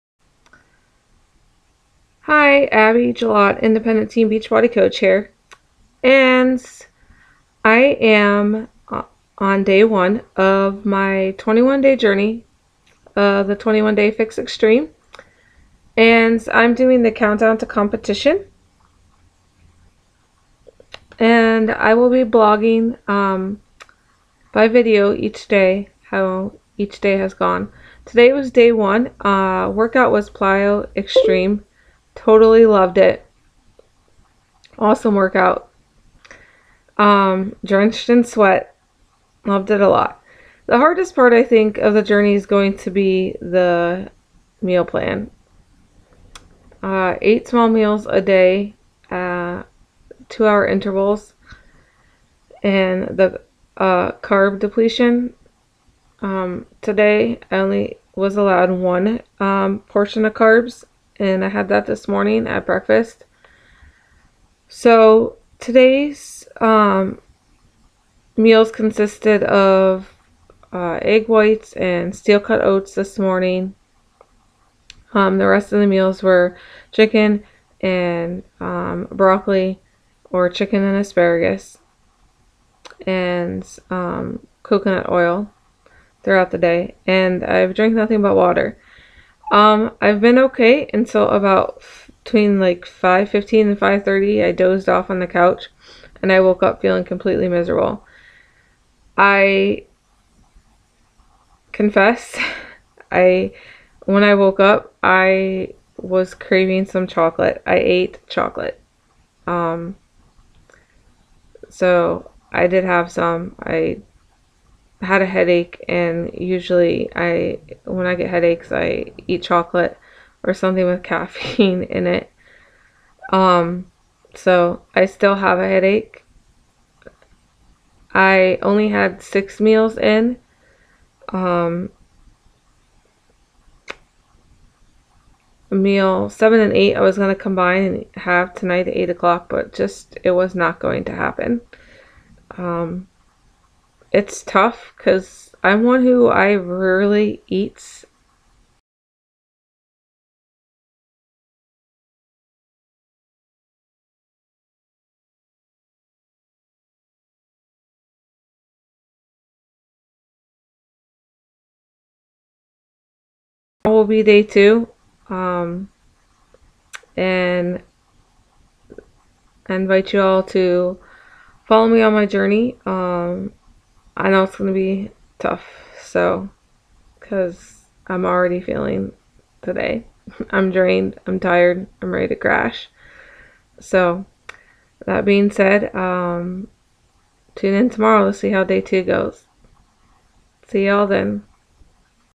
21 Day countdown to competition Day 1
21 Day journey of 21 day fix extreme countdown to competition. Follow my journey as I take the next 21 days and follow a strict plan to competition I am not doing a competition but I will be following the plan as if I am going to be doing one.